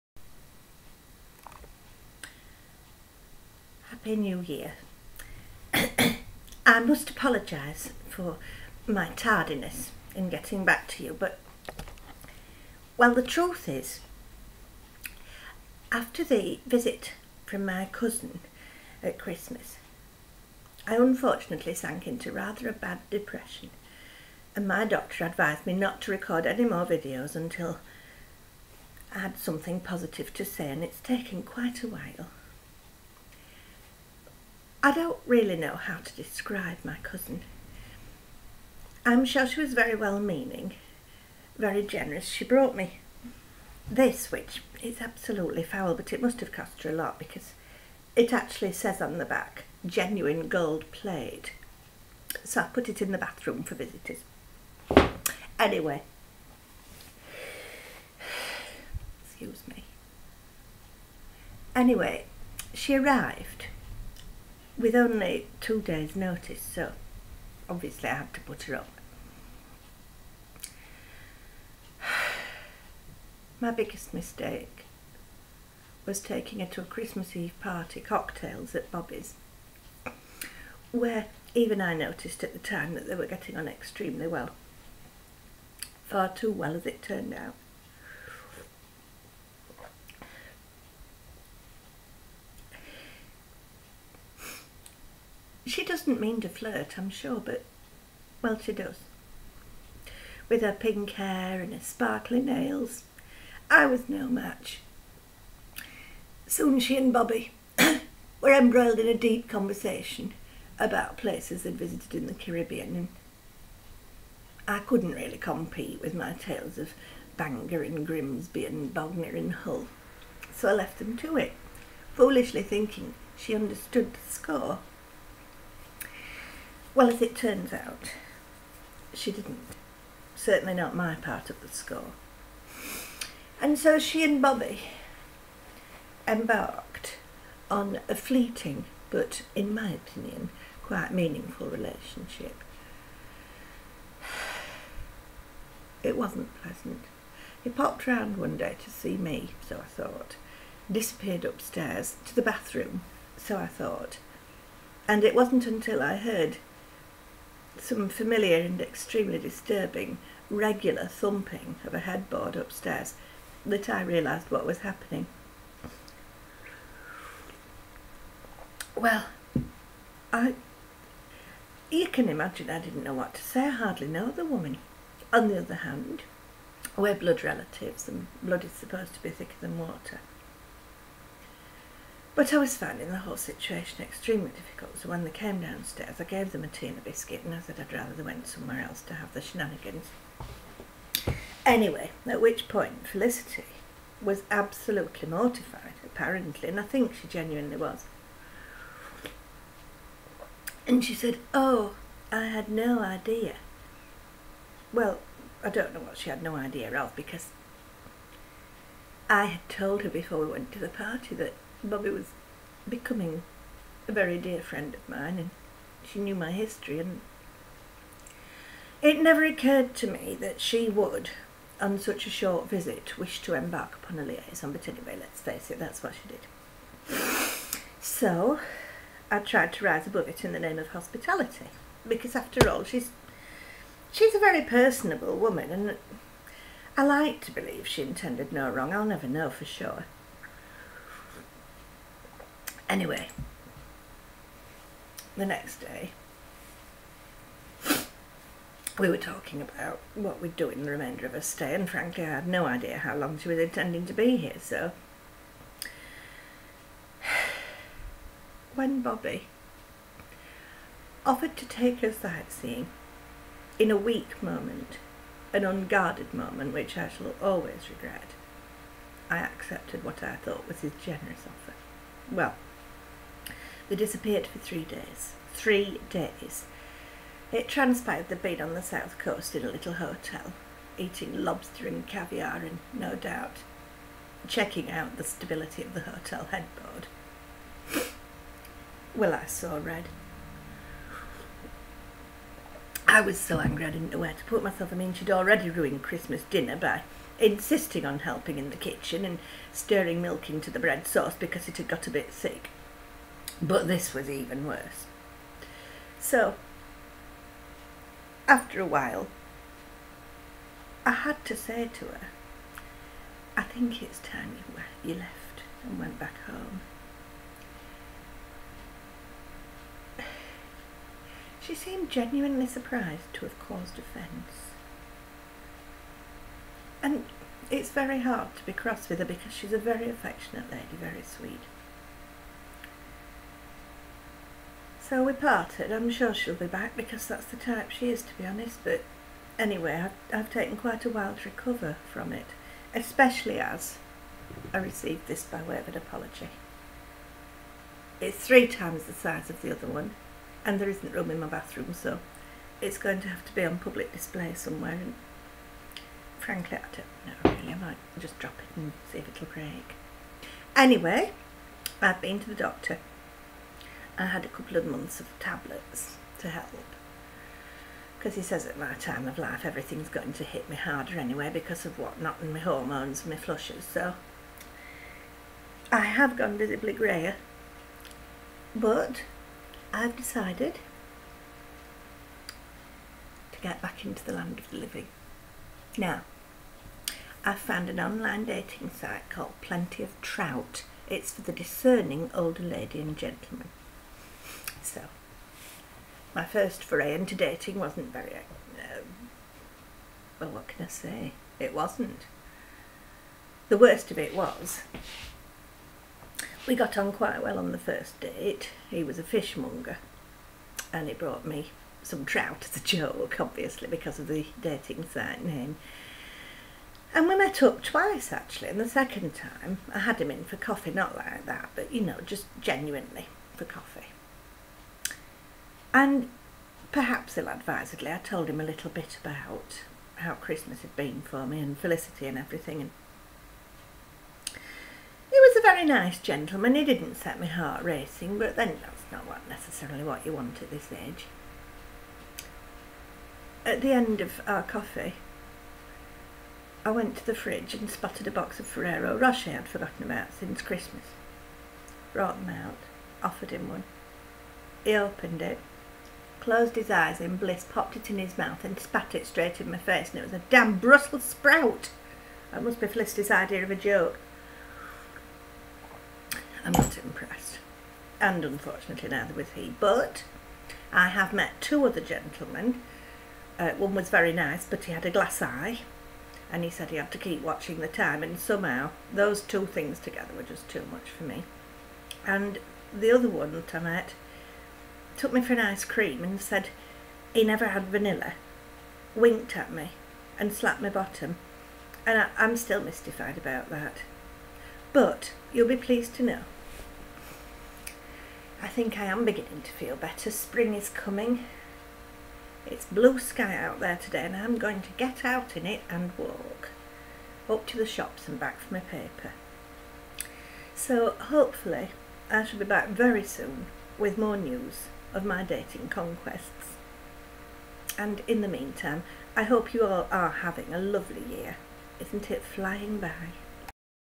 Getting Over Christmas...
Edna is still in recovery from her cousin Felicity's visit.